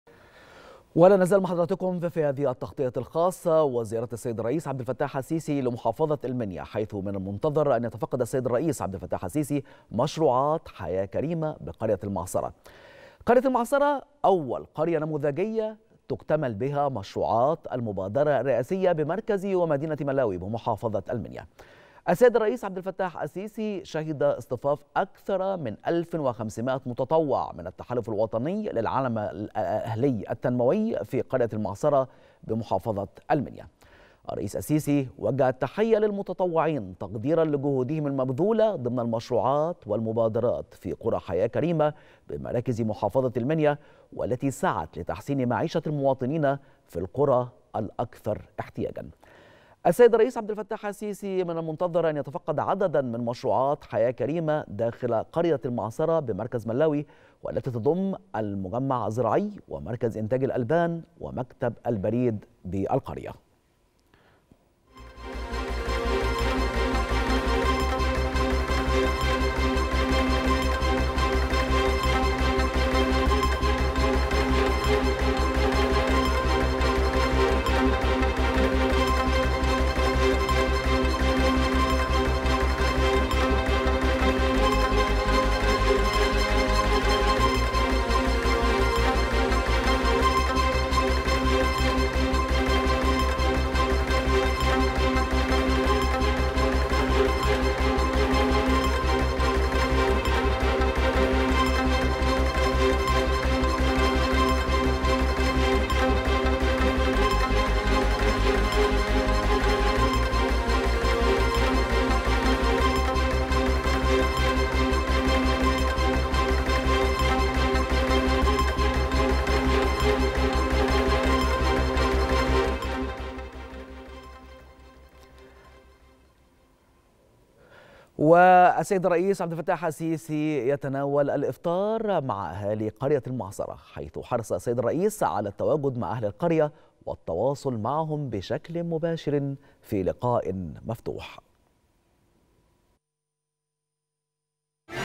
الرئيس السيسي يتفقد مشروعات حياة كريمة بقرية المعصرة والتي تضم المجمع الزراعي ومركز إنتاج الألبان
تغطية خاصة | الرئيس السيسي يتفقد مشروعات حياة كريمة بقرية المعصرة والتي تضم المجمع الزراعي ومركز إنتاج الألبان ومكتب البريد